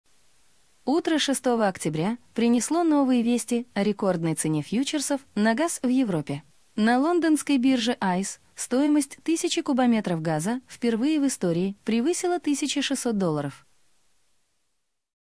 Новости политики .Новый рекорд: газ в Европе уже по $1600!
. Новости политики .«Комсомольская правда» уходит из Белоруссии

 – новости политики.

2 дня назад - В среду, в вечернем эфире «Беларусь 1» появилось новое ток-шоу «Понятная политика плюс»  - 60 минут прямой эфир: